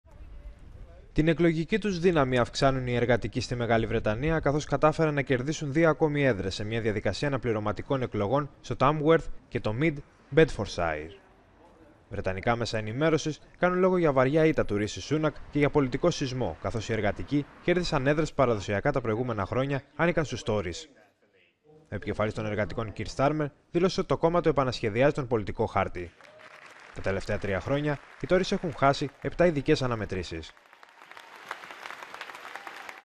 Βρετανία: Βαριά ήττα για τους Συντηρητικούς σε αναπληρωματική εκλογική διαδικασία…
Η υποψήφια των Εργατικών Σάρα Έντουαρντς κέρδισε την έδρα του Τάμγουερθ, στην κεντρική Αγγλία